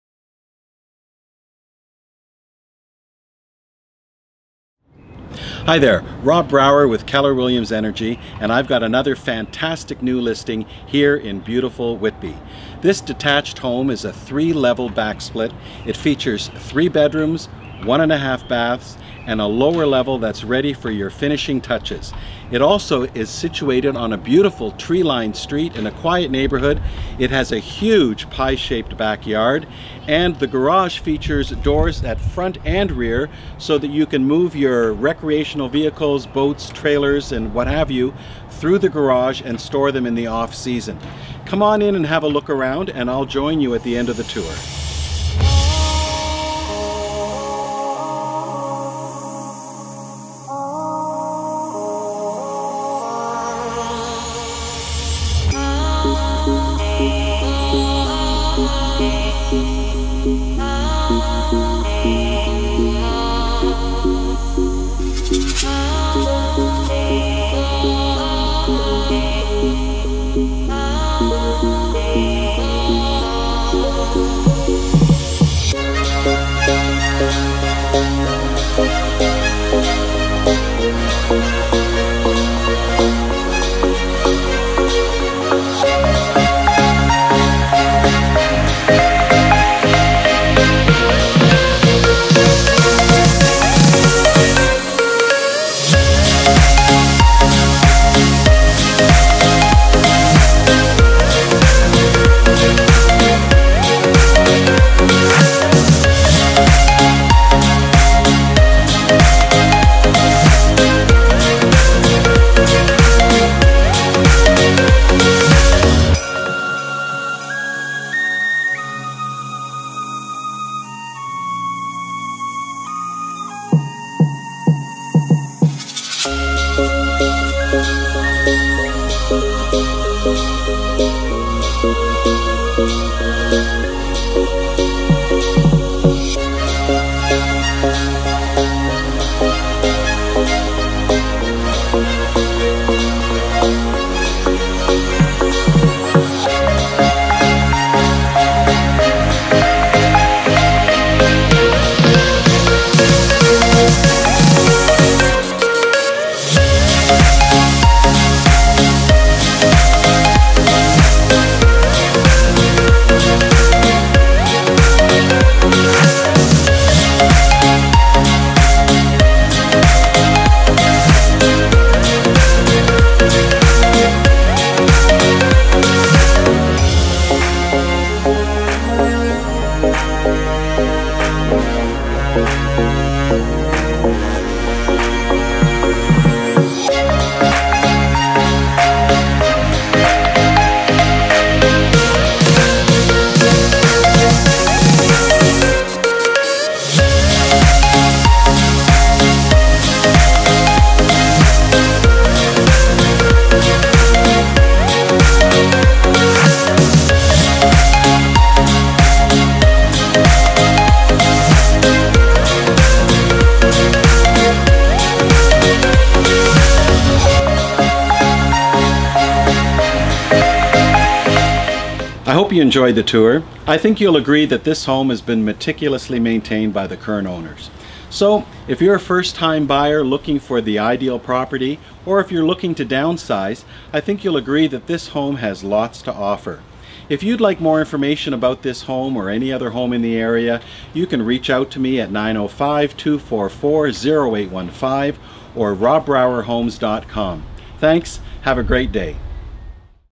140 Calais St Branded
Move In Ready! Meticulously Maintained 3-Level Back-Split On A Quiet Street In A Mature, Highly Sought-After Whitby Neighbourhood. Huge, Pie-Shaped Yard, Lush Landscaping. Drive Through Garage Handy For Storing Your Boat/Trailer In The Off Season! Generously Sized Bedrooms. Master With 2 Pc Ensuite and Double Entry Doors. This Home Shows Pride Of Ownership! Lower Level Has Separate Entrance. large, above grade windows and is Ready For Your Design! Create an In-Law suite, or more family space!
Extras: Includes Fridge, Stove, Washer & Dryer & Alarm System. Freshly Painted. Updated Windows, New Front Windows'15, A/C & Furnace '06, Roof '10, Garage Door '14 10 Minutes To Go Train. Easy Access To 401  For more information or to book your private showing, contact the Listing Agent, Rob Brouwer